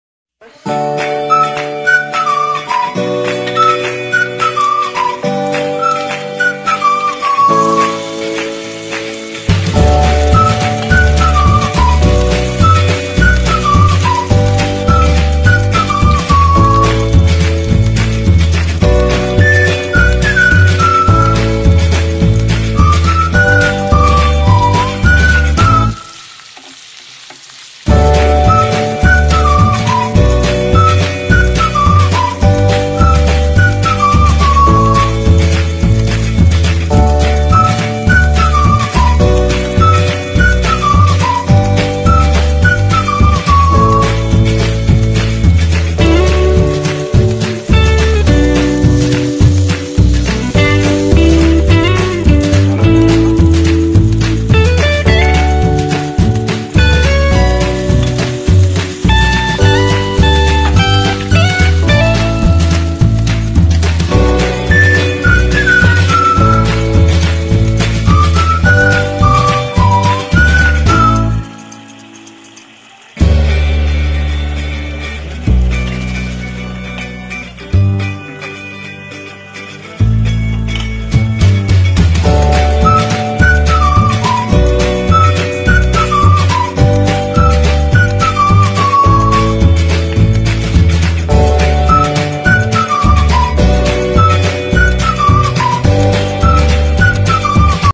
रेलवे वाली आलू की सब्जी सिर्फ 1 मिनट में बनाना सीखें | Railway wali Aloo Ki Sabji | 1 minutes Recipe
I had Taken 4 BOIL potatoes